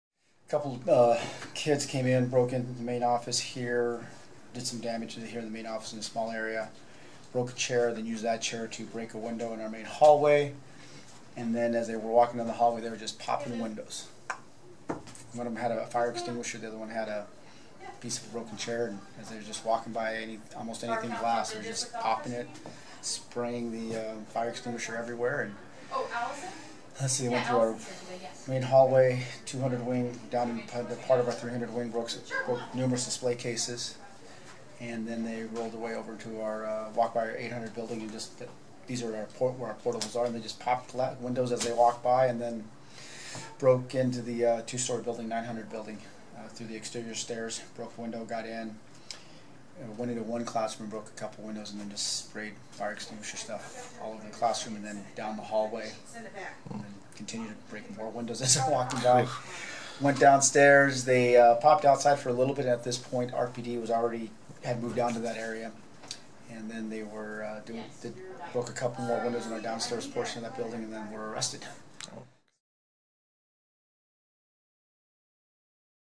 Shasta High principal describes vandalism to school
Shasta High School Principal Leo Perez describes how two teens broke into and vandalized his school early Friday. Perez's account is based on surveillance footage at the school.